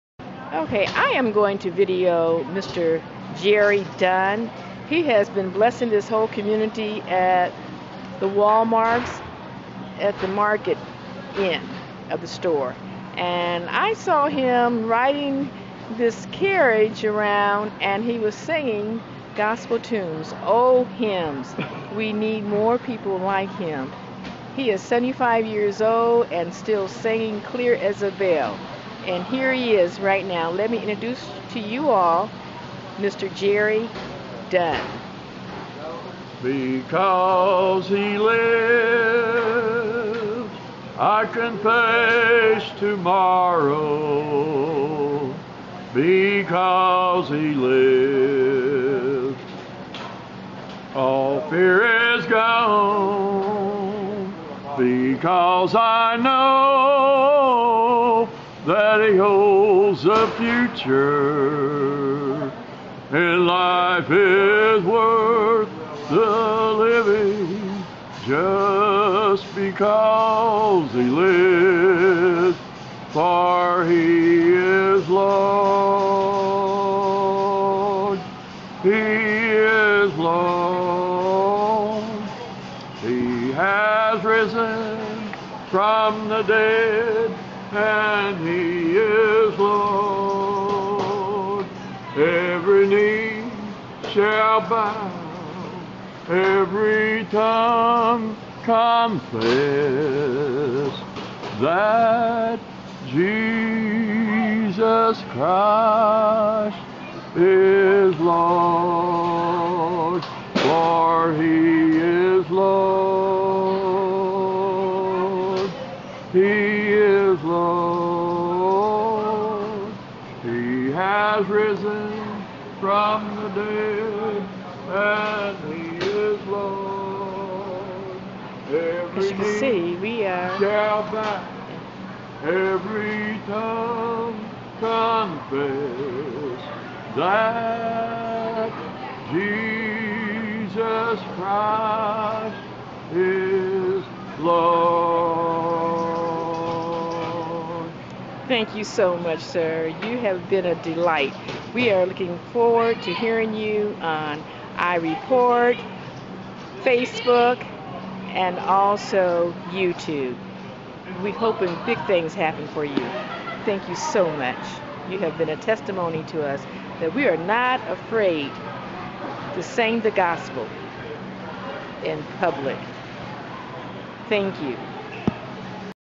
PT#1 MR. JERRY DUNN - A 75 YEAR OLD MAN SINGING OLD GOSPEL HELM'S IN WALMARTS
A seventy-five year old man singing in Walmart's off the 80fwy. singing gospel helm's while people shop.   Everyone was definitely enjoying Mr. Dunn's strong voice, he did not  miss a key.   It's was refreshing to see as people was calm by Dunn's singing Holy spirit.